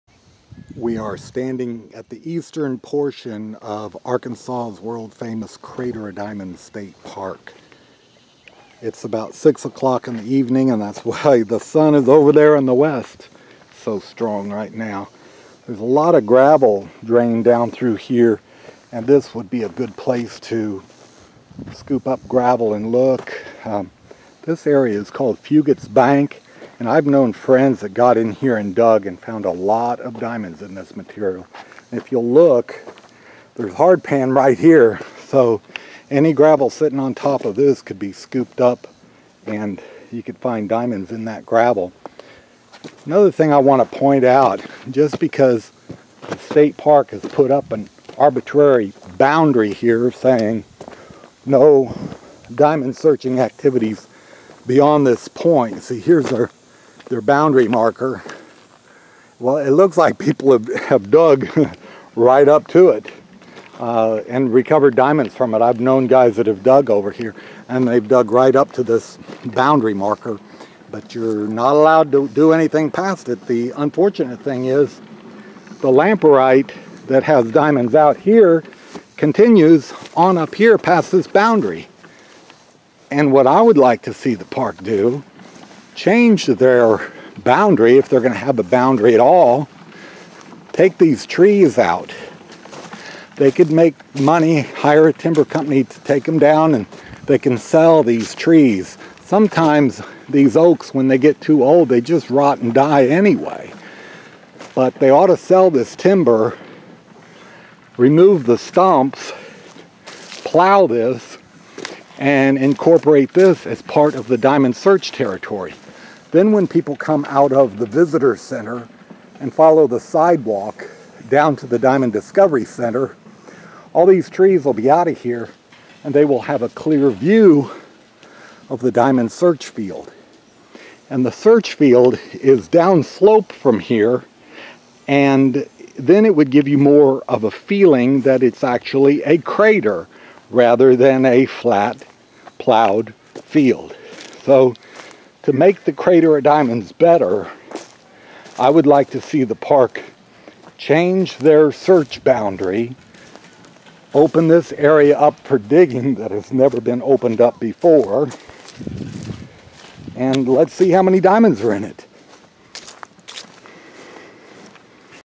New Search Territory Boundaries Should Include ALL of Fuggitt's Bank
Diamonds have been found right up to the east boundary line at Fuggitt's Bank.  The geologist's map shows the diamond bearing soil continuing on up the hill almost to the sidewalk between the Visitor's Center and the Diamond Discover Center.  I propose that the boundary markers be moved and this section be included in the Crater of Diamonds search territory.